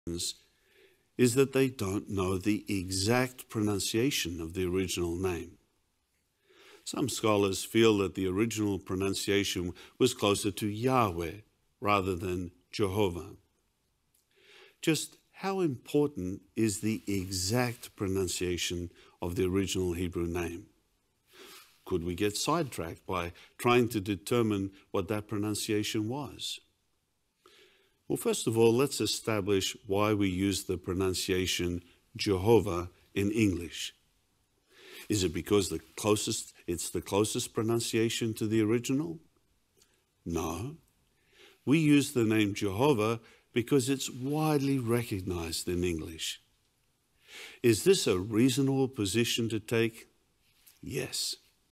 Jehovah is NOT the Accurate Name of God
Another clip from one of my videos, meant for sharing with Jehovah's Witnesses.